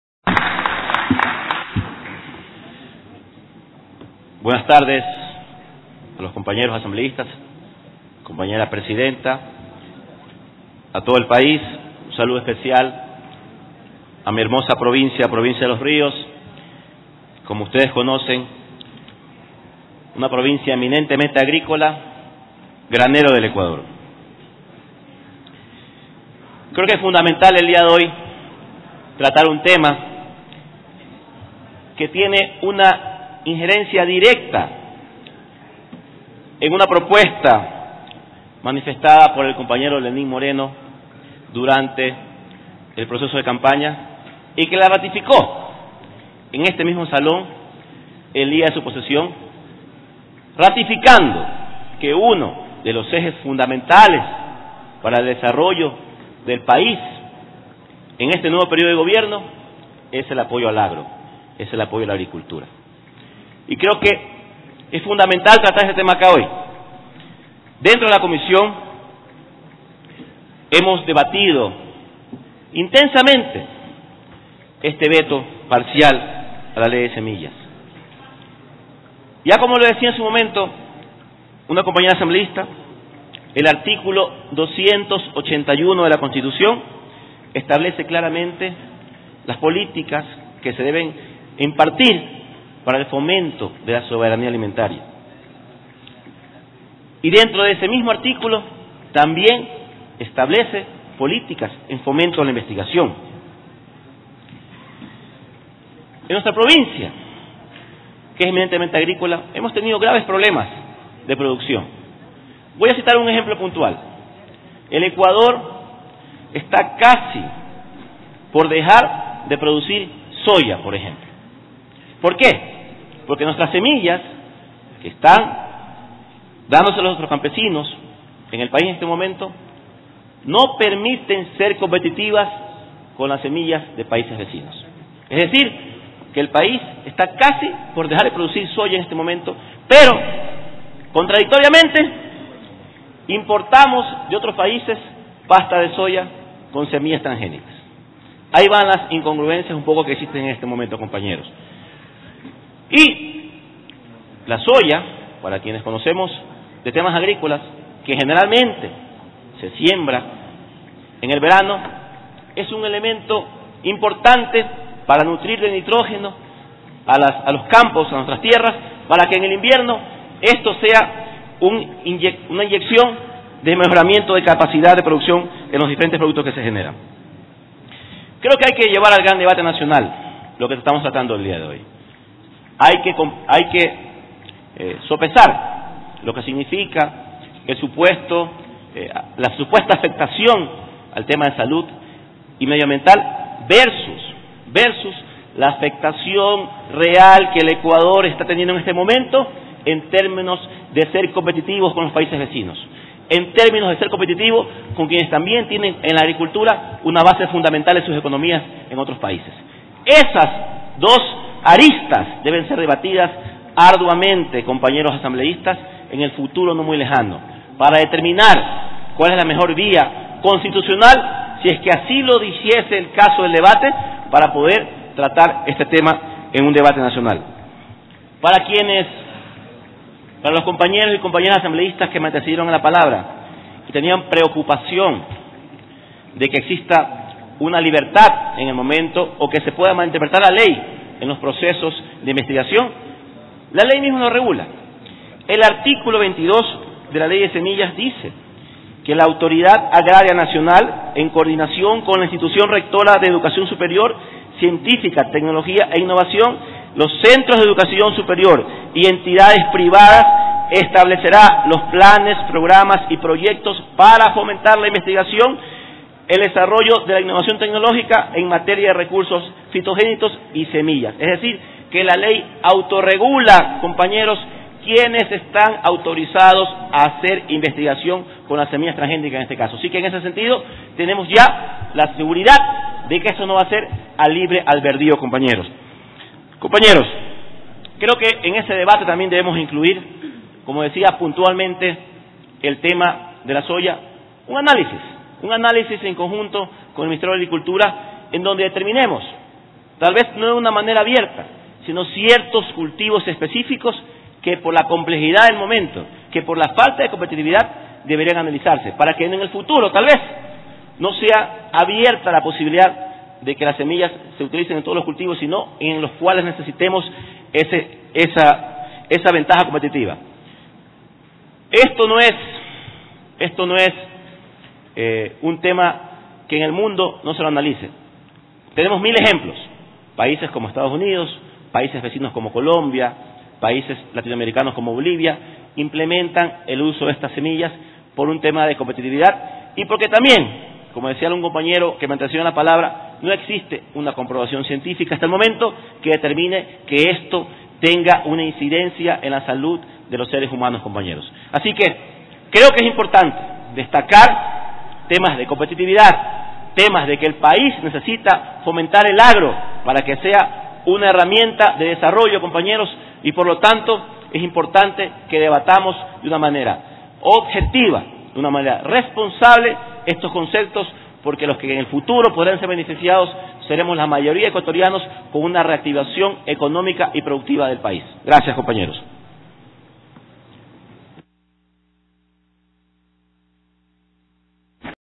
César Litardo - Sesión 457
01 de junio de 2017
Objeción Parcial al proyecto de Ley Orgánica de Agrobiodiversidad, Semillas y Fomento de la Agricultura Sustentable.